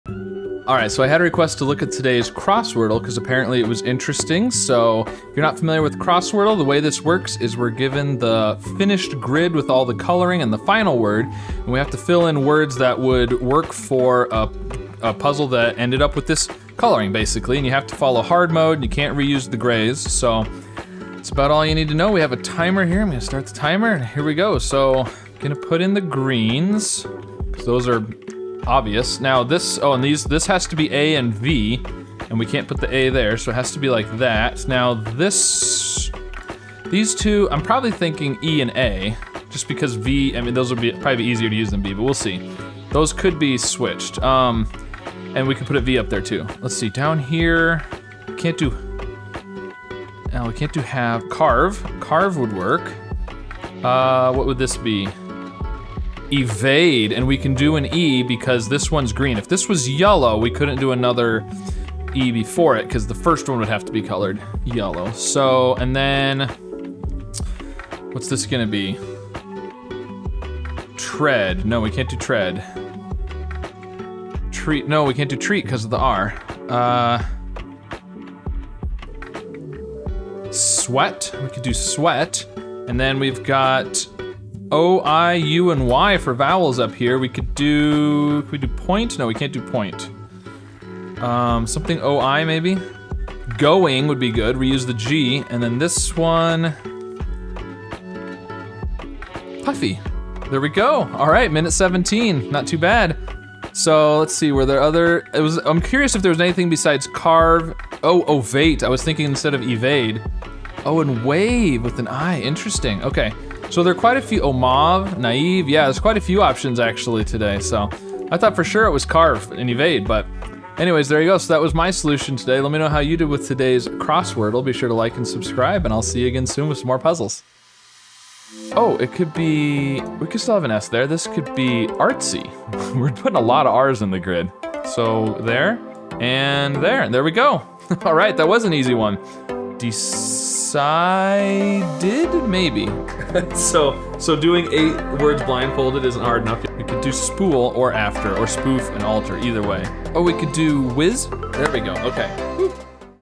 Can you find the pressure release VALVE on this puzzle?
I'm Scott Strosahl, and today I'm solving the Crosswordle for September 11th.

Crosswordle is kinda like Wordle in reverse... check out my previous solve of several crosswordles to learn more about the rules